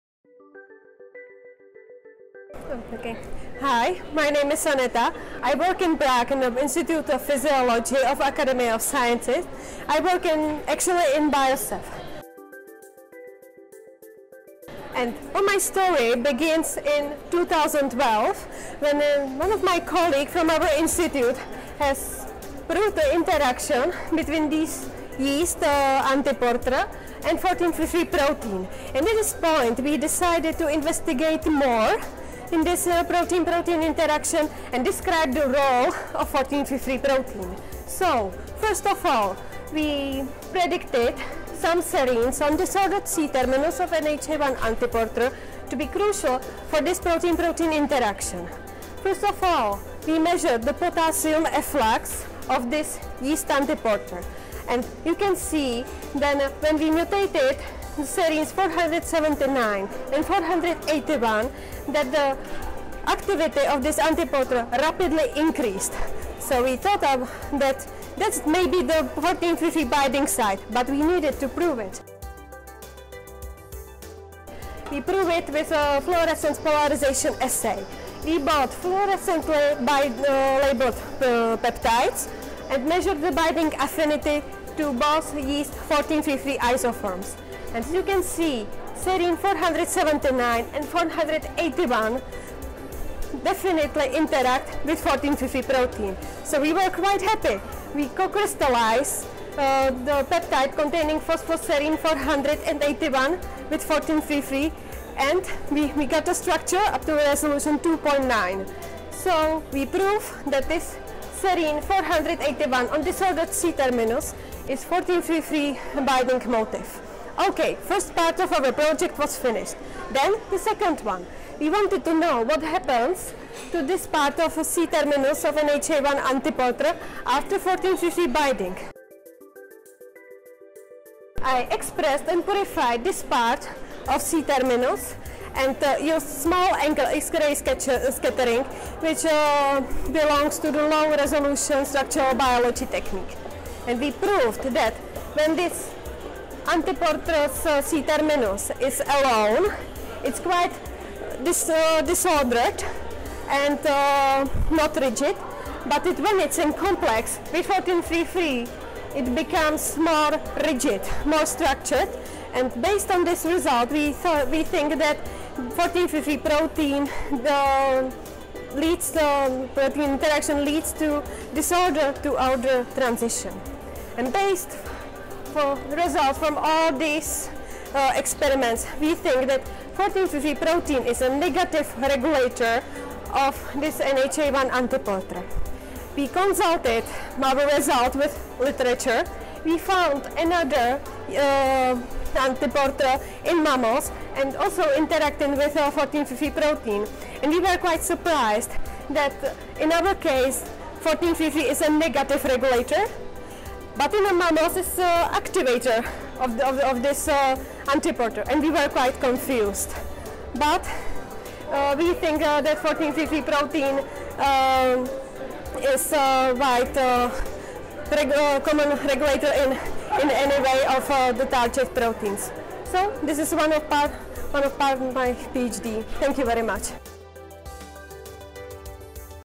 FEBS 2019 Posters- Aneta Smidova
Aneta Smidova, Institute of Physiology Academy of Sciences, Czech Republic, tells us about her studies into the Nha1 antiporter 14-3-3 complex